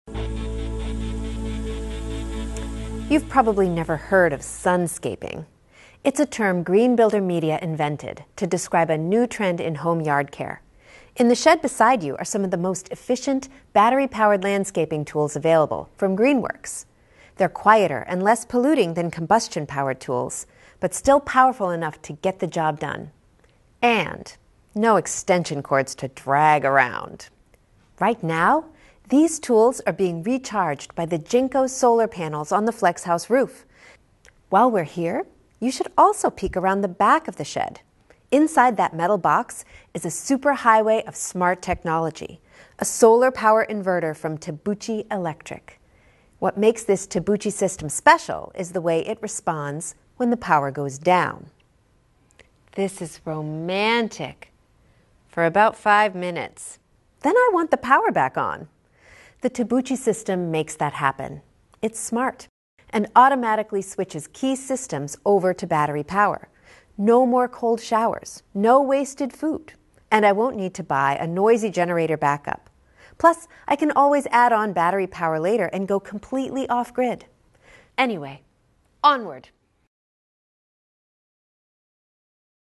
Sun-Scaping at the Flex House
Sun-Scaping, a term coined by Green Builder Media, refers to the new trend of operating battery-powered landscaping tools that are recharged with solar power. This is part 1 of 12 of the Flex House video tour. The final stop for the exhibition home, created by Green Builder Media and Shelter Dynamics, is CES Las Vegas, Jan. 9-12, 2018.